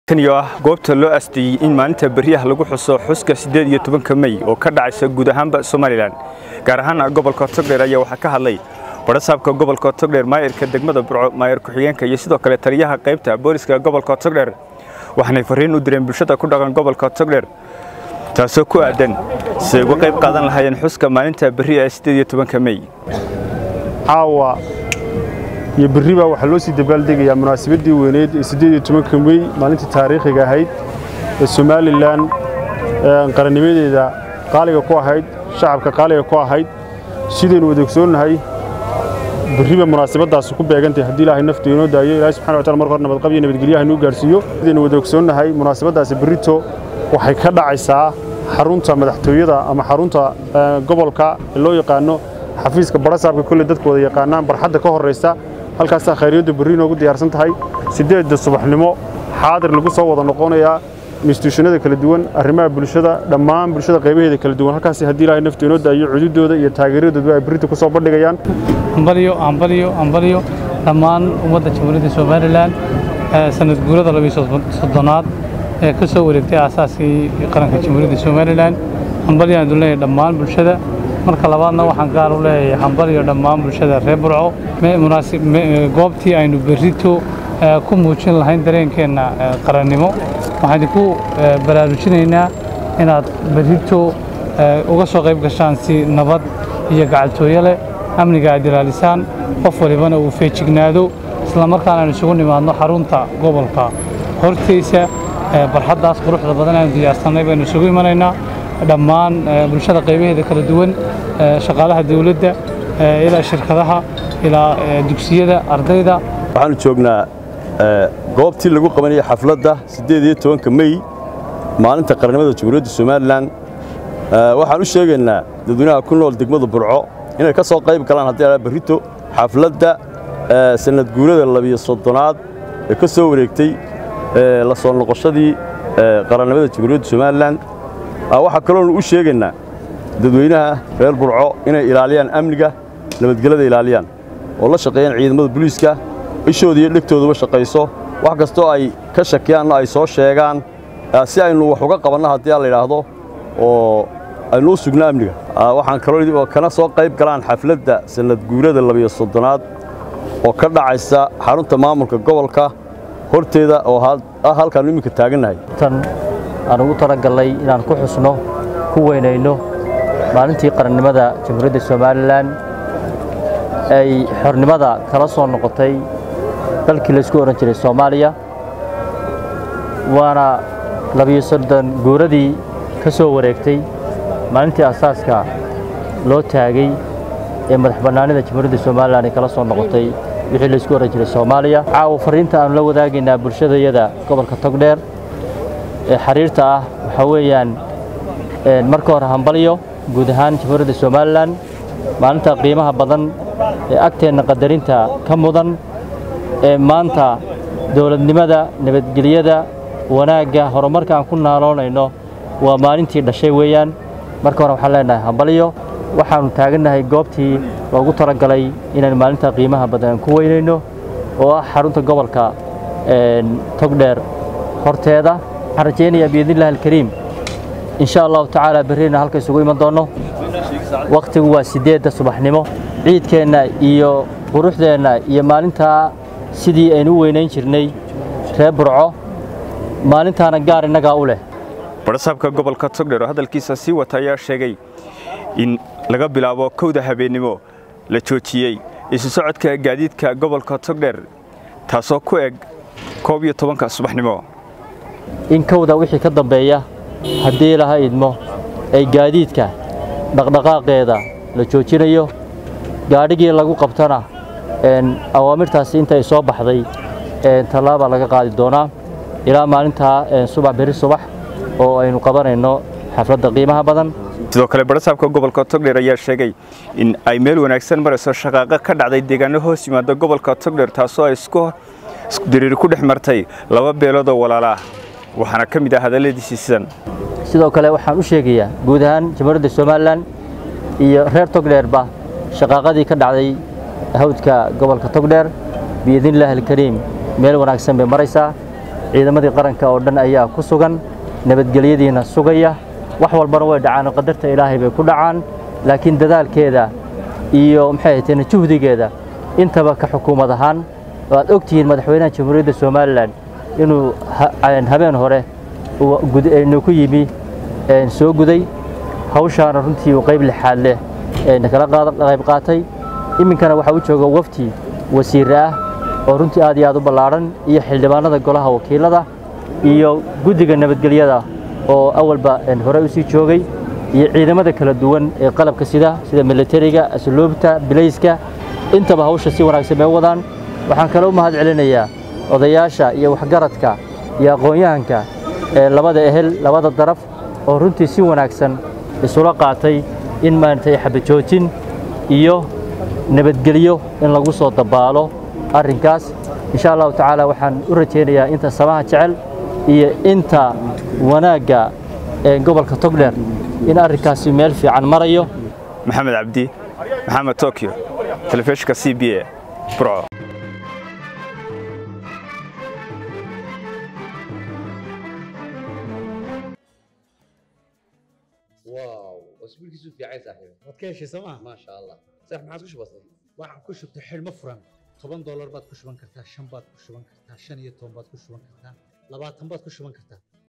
1da habeenimo laga bilaabo magaalada burco waa bandow guddida qaban qaabada Burco ee xuska 18 May
1da habeenimo laga bilaabo magaalada burco waa bandow guddida qaban qaabada Burco ee xuska 18 May